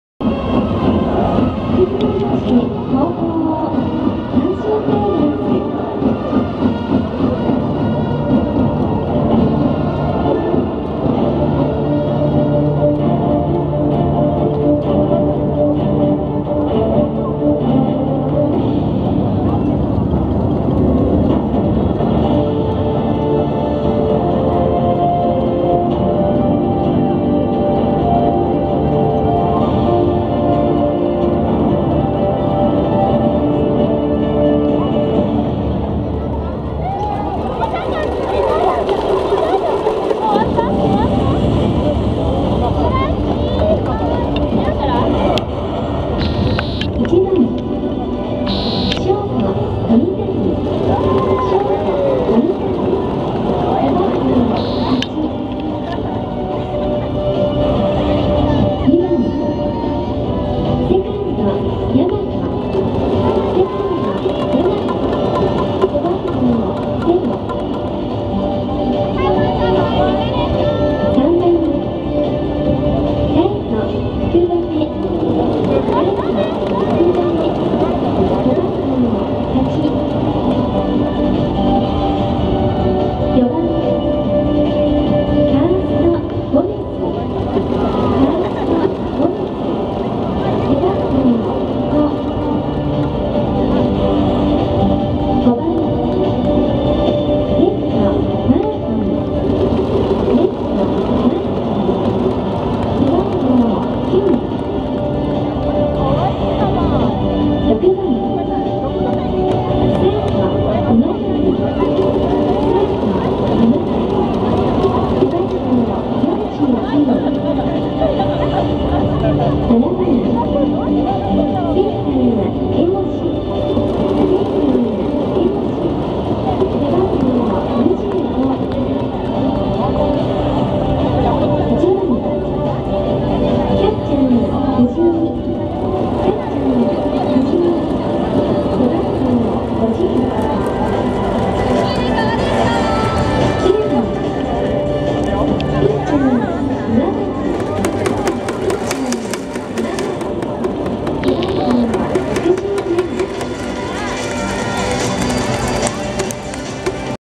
20150822 阪神vs横浜DeNA 阪神スタメン発表
京セラドーム大阪ライト上段席より撮影

1.鳥谷
2.大和
3.福留
4.ゴメス
5.マートン
6.今成
7.江越
8.藤井
9.岩崎